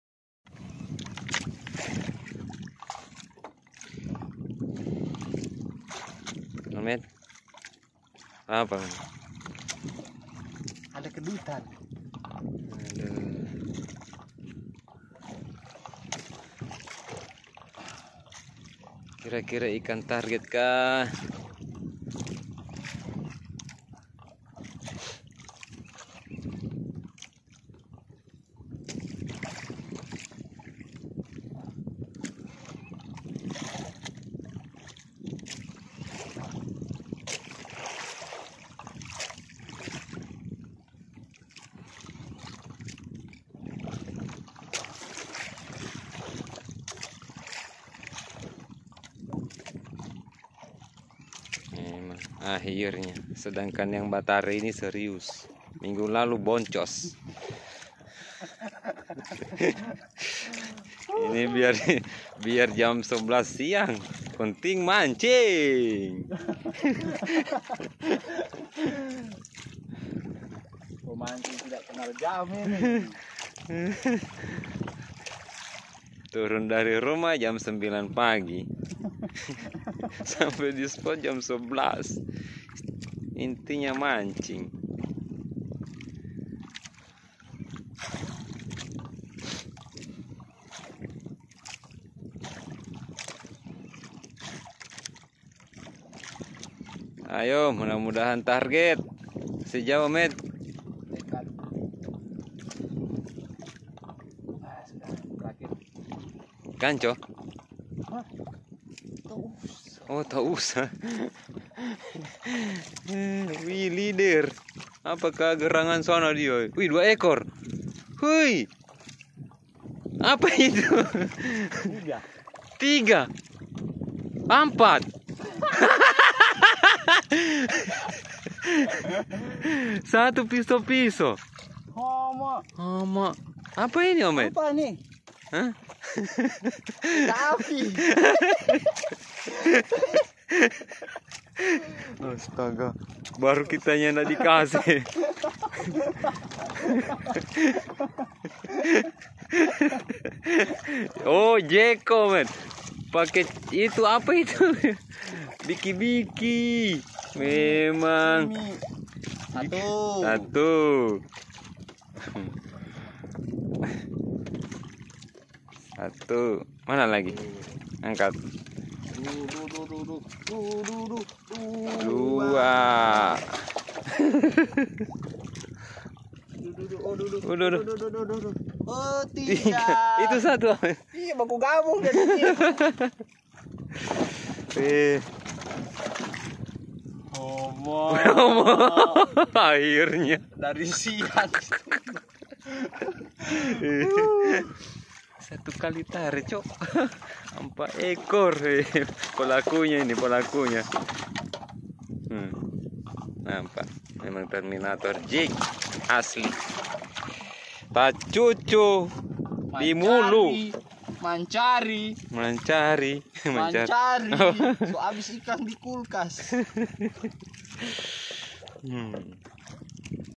WOWWW🎣🐟💯 1 ROD 3MATA BONGSANG SAMA YANG PUNYA @TERMINATOR JIG👍
Untuk moments nya ke video tanpa diduga teman yang punya pabrik Terminator Jig Palu sekali turun kan jig langsung dapat 3 ekor ikan mata bongsang.
siapa tahu ada yang minat jig Terminator hub FB Meidy Jig's Poso